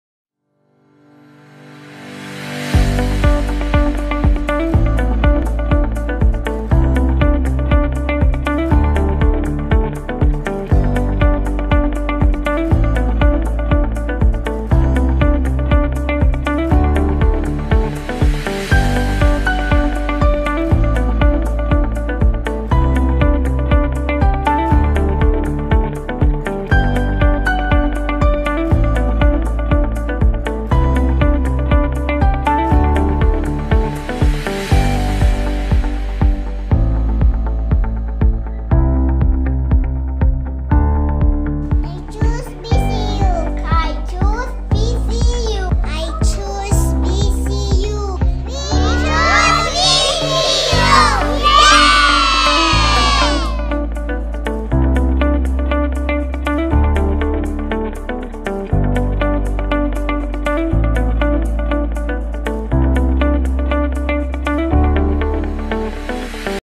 BAGUIO CENTRAL UNIVERSITY (BCU) GROWING UP 2018 AVP
A stopover to " GROWING UP LEARNING CENTER " , a highly recommended school for PRESCHOOLERS AND KINDERGARTEN AGES 4-6 YEARS OLD - situated at lower Padre Burgos, Baguio City.
QUALITY EDUCATION and ABSOLUTELY LOW TUITION FEES at BAGUIO CENTRAL UNIVERSITY ! ENROLLMENT SCHEDULE FOR COLLEGE DEGREE PROGRAMS FOR THE 2ND SEMESTER BEGINS ON JANUARY 3-14,2019.